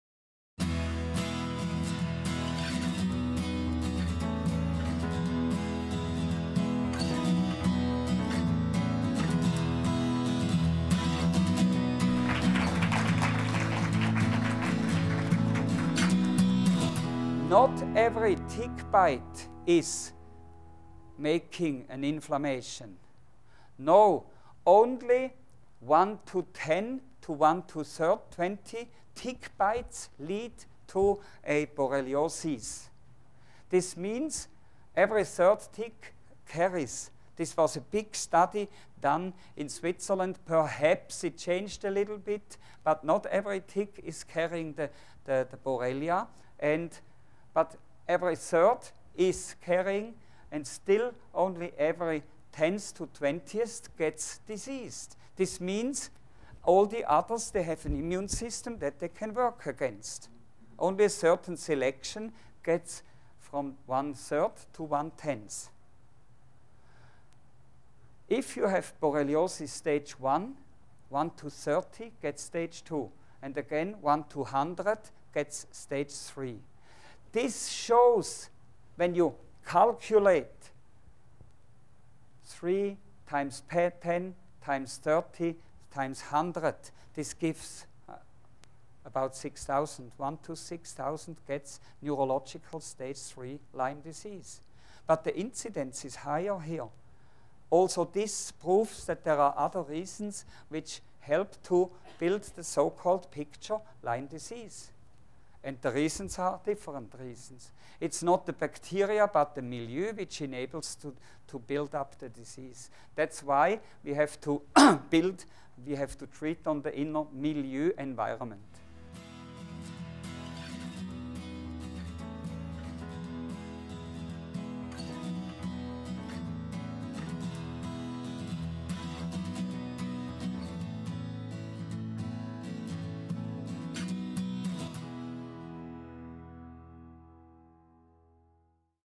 Metrics of Tick Bites and Borrelia
In an excerpt from his talk in Tysons Corner, Virginia, Swiss Physician Dr. Thomas Rau breaks down the numbers relating to tick bites and Lyme disease.